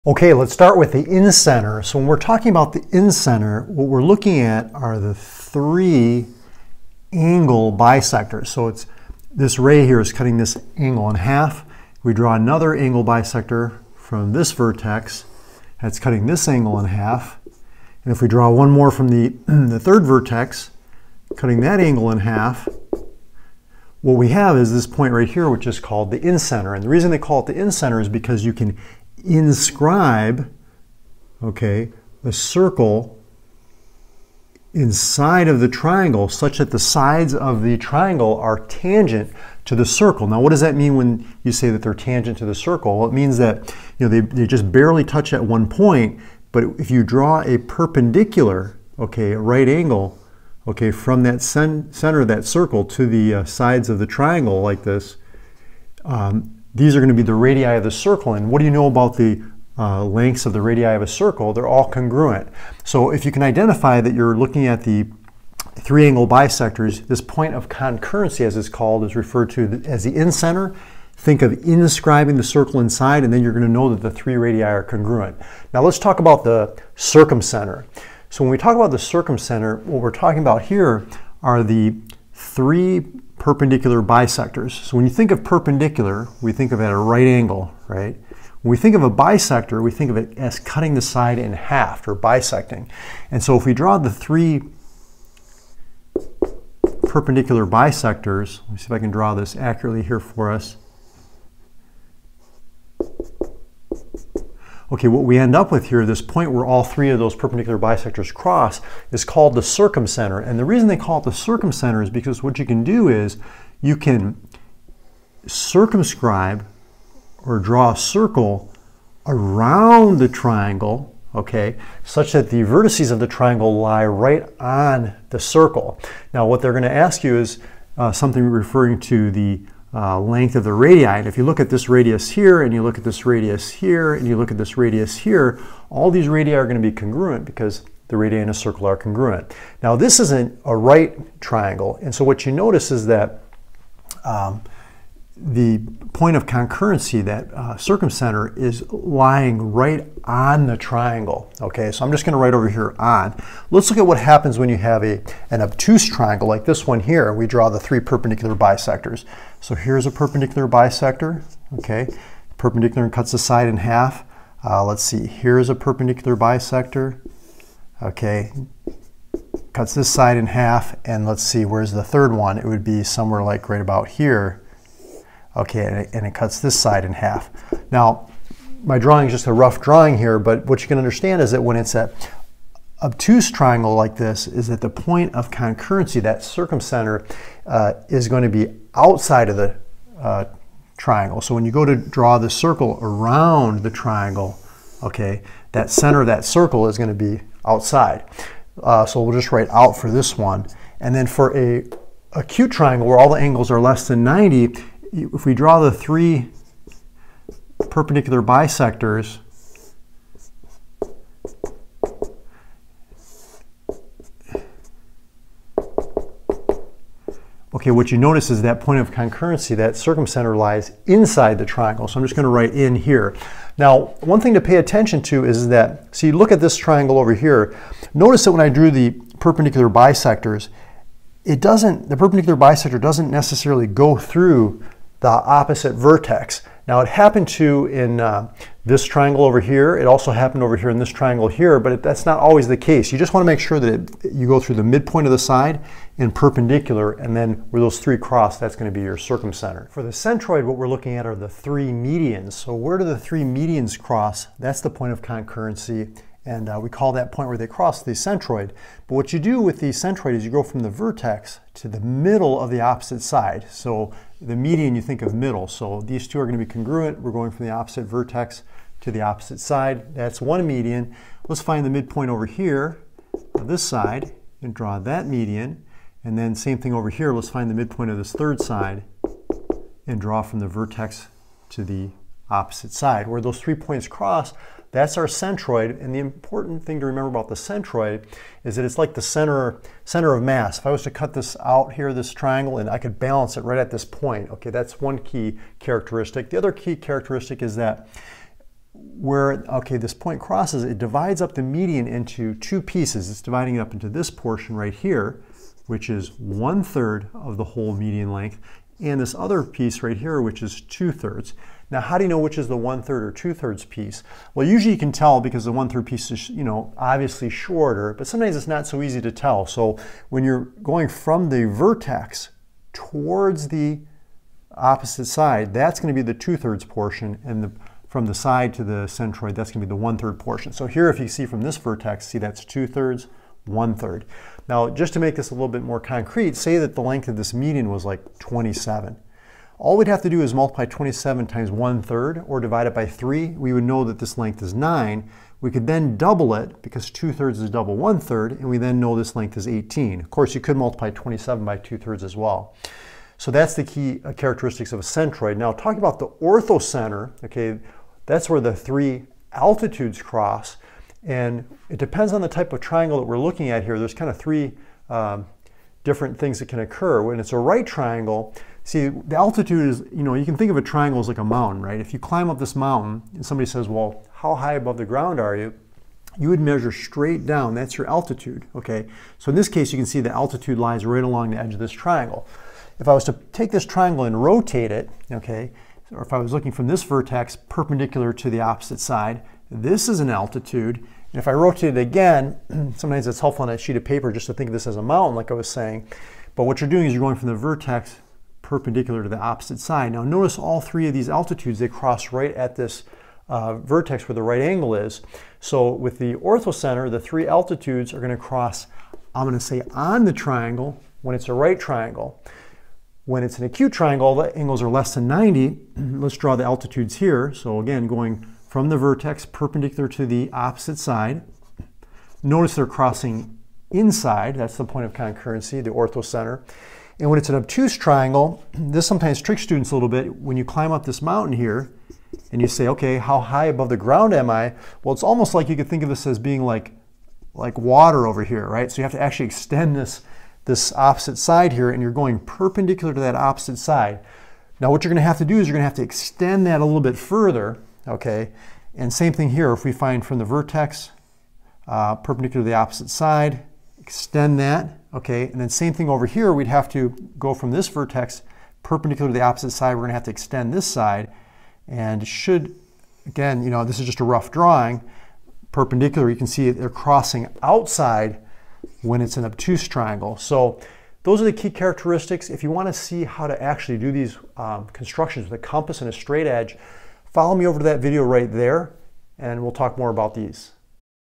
Incenter, Circumcenter, Centroid, Orthocenter (Properties & Diagrams)
Learn what the incenter, circumcenter, centroid and orthocenter are in triangles and how to draw them.  We discuss these special points of concurrency in this math tutorial by Mario's Math Tutoring as well as the special properties of these points.